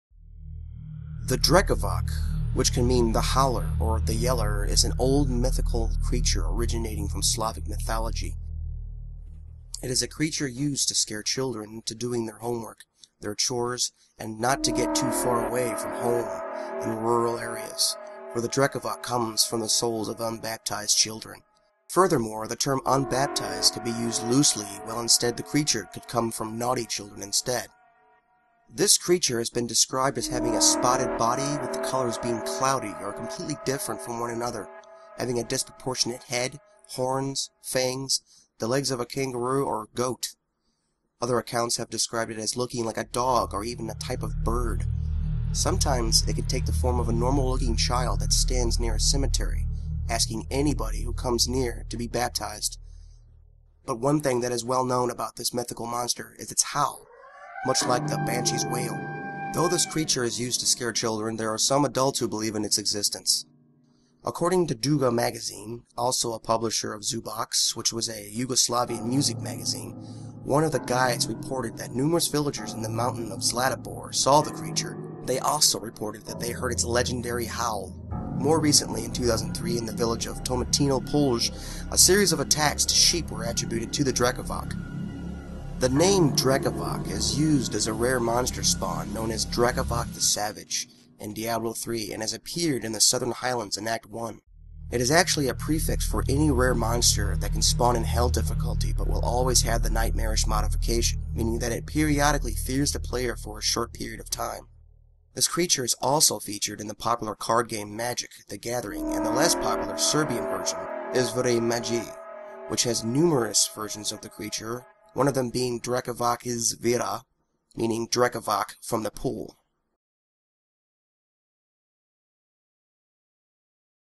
Drekavac - The Undead Encyclopedia
This episode is about the Slavic creature spawned from the deaths of unbaptized children.